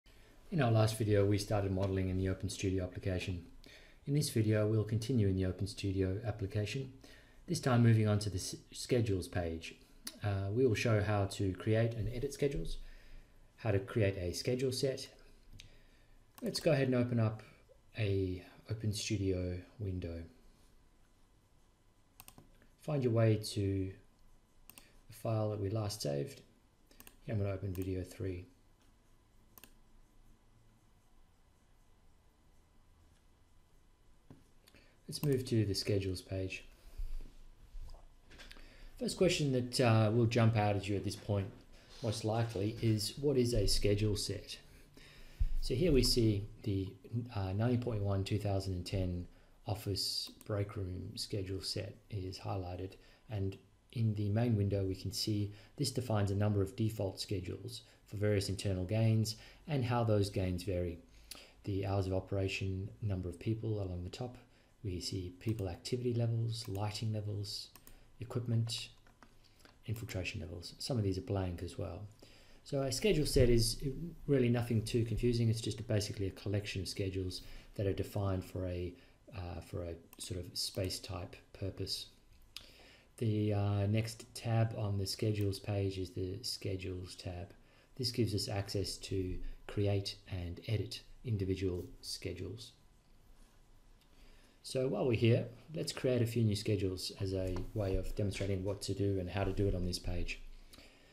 4 - Managing Schedules in OpenStudio
Let’s add three new schedules.
Hit the green plus button
Choose Fractional
Name this Always On
Now let’s create an Always Off
Let’s add a schedule that represents the overall use of electrical equipment in the building through the working week.
Add a new fractional schedule
From 8-5 this can be all on, after hours, we set it to be 15% on
How to create and edit a schedule set
Let’s add a new Schedule set.
Hit the green button and name this My Schedule Set
Add the electrical equipment we created and a few others
Note the schedules we created are visible under My Model
There are also schedules under Library
We can access some information on the Schedules under Edit.
That is a brief overview of the Schedules page. As we a using a set of Schedules for the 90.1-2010 performance method, there is not much work todo here.
Save your file here as Video4.osm in the next video we will move on to the Constructions Page.